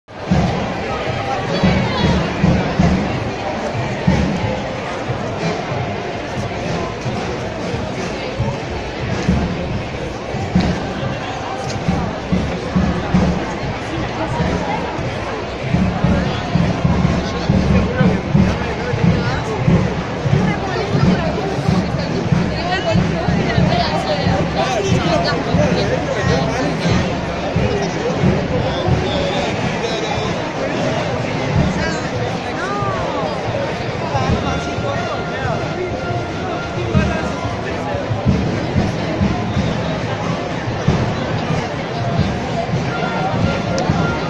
niunamenos - Córdoba
Movilización contra la violencia machista - Na tarde de 19 de outubro, milhares de mulheres argentinas vestiram preto e pararam de trabalhar durante uma hora, das 13h às 14h. Depois, às 17h, seguiram em marchas organizadas em várias cidades do país.
Córdoba, Argentina, 19 outubro 2016
Registro: Júlia Coelho